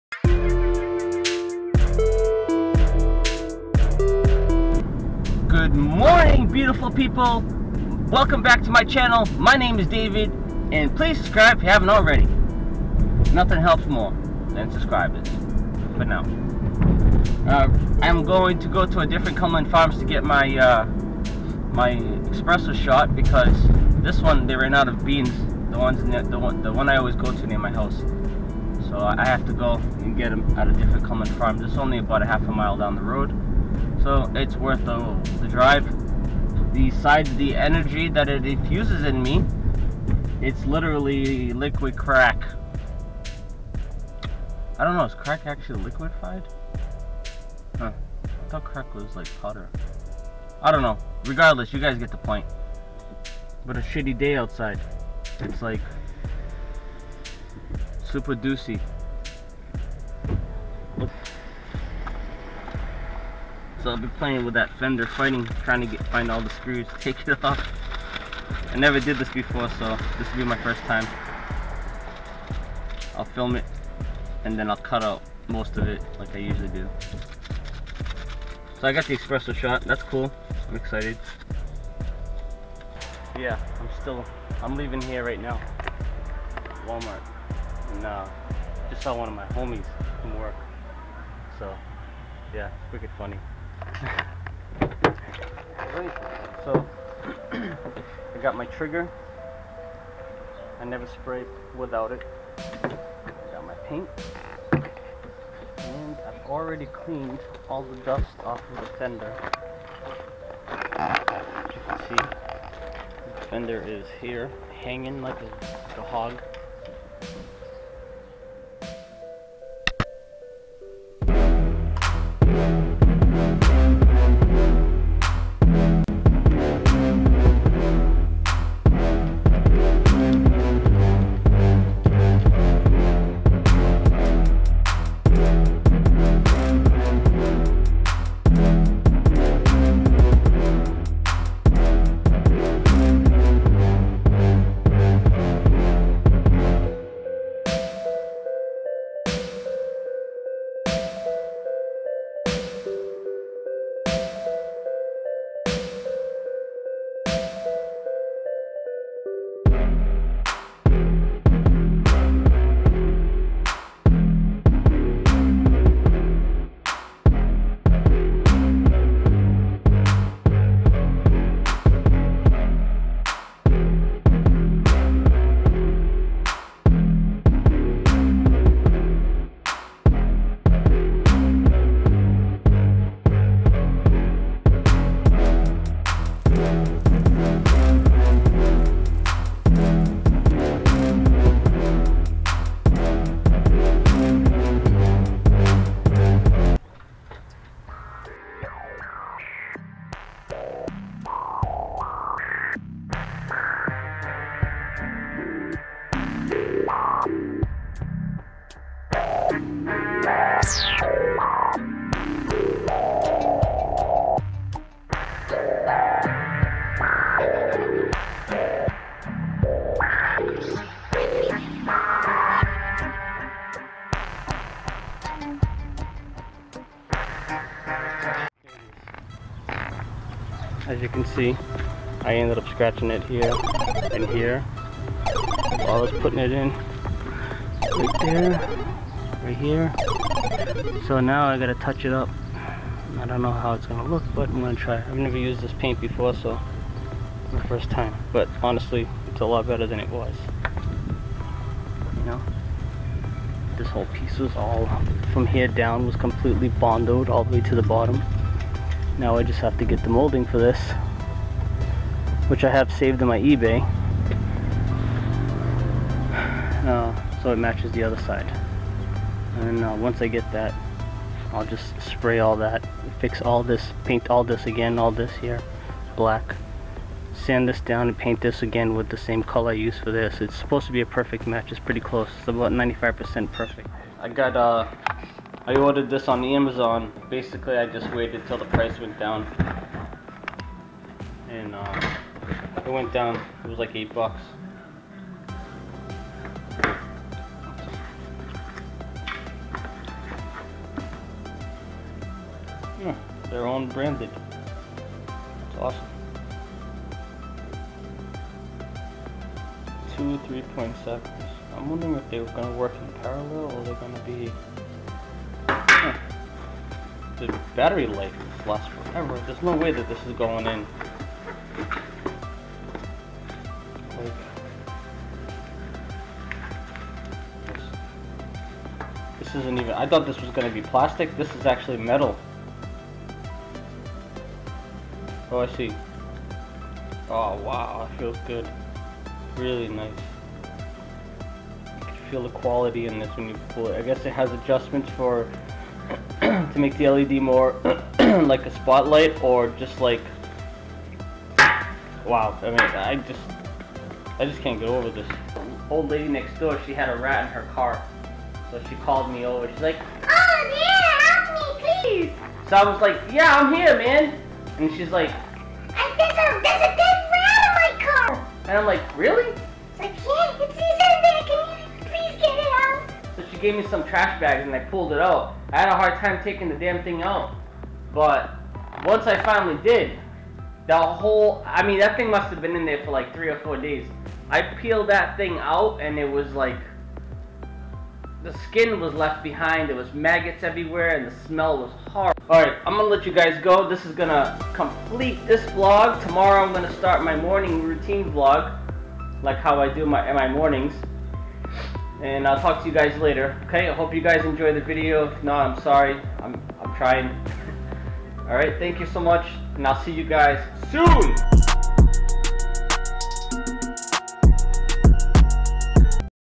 DIY equinox Spray panting + Install coffee Tacklife headlamp VLOG 13
DIY equinox chevy 2010 Spray panting Fender + Install
Morning coffee
Tacklife headlamp first Impressions
VLOG 13